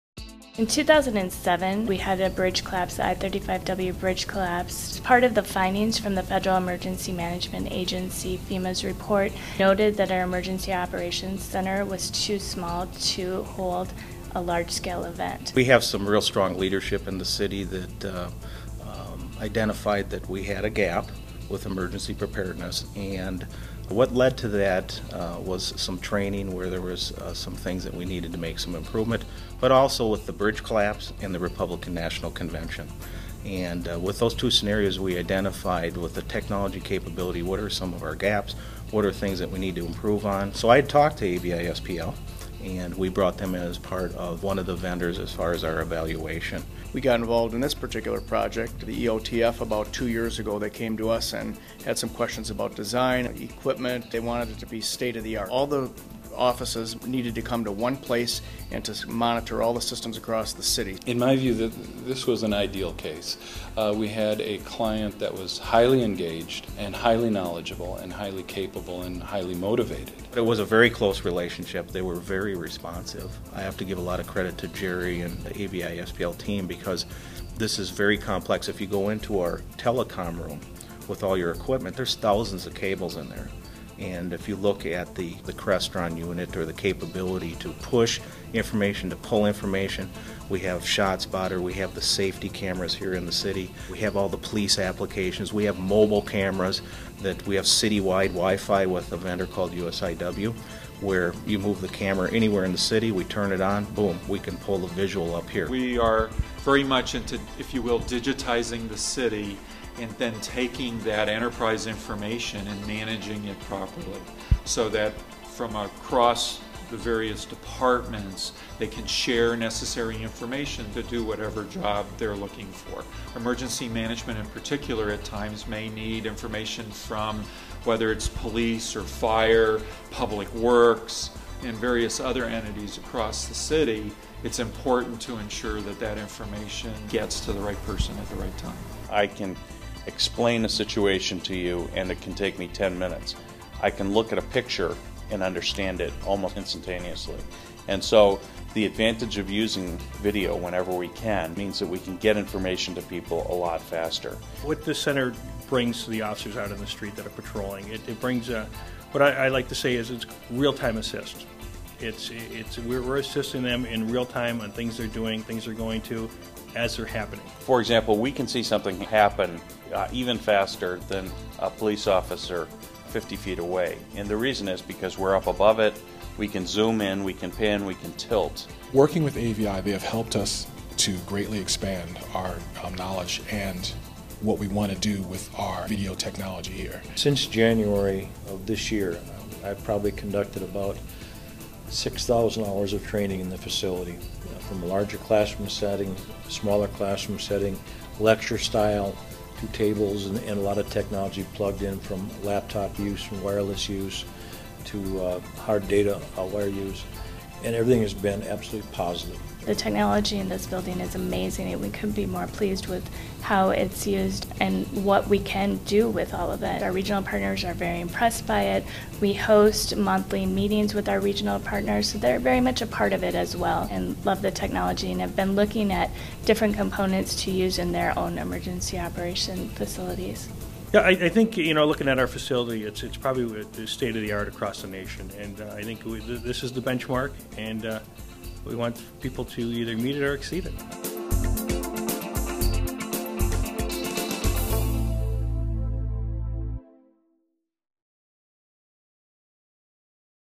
Minneapolis Improves Emergency Operations with Advanced Technologies
In collaboration with AV leader AVI-SPL, the city of Minneapolis has dramatically improved response time and monitoring of emergency operations. Innovations include video conferencing capabilities, projection wall technology for increased surveillance, and digital system control and management.